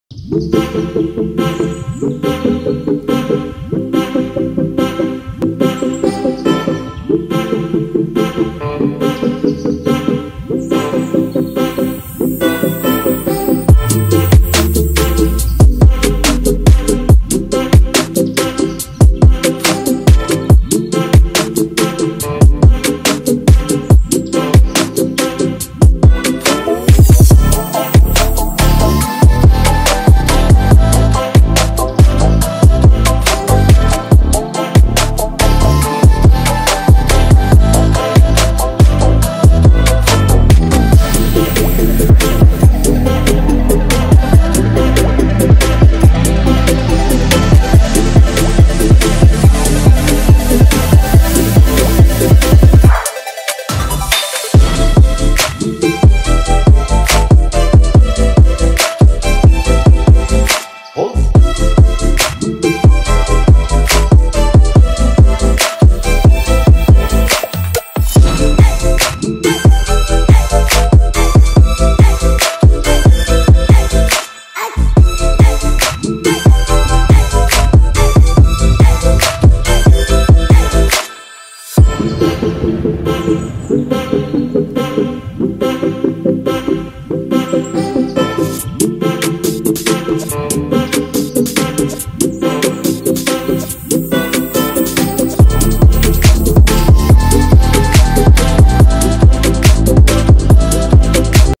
Places to visit near Chitradurga|| Davangere to Chitradurga trip||1day trip part wise
@Kadamba .Places to visit near Chitradurga
Plan Your trip to Chitradurga in a convenient way
1 day  trip plan  from Davanagere